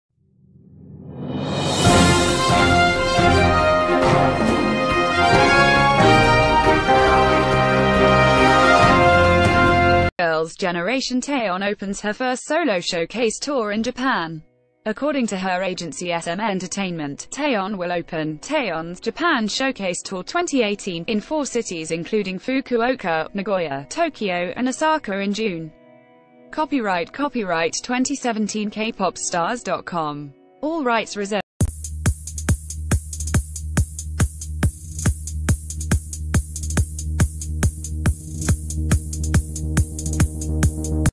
Taeyeon to hold its first stand-alone showcase tour in Japan in June
Girls' Generation Taeyeon  opens her first solo showcase tour in Japan. According to her agency SM E...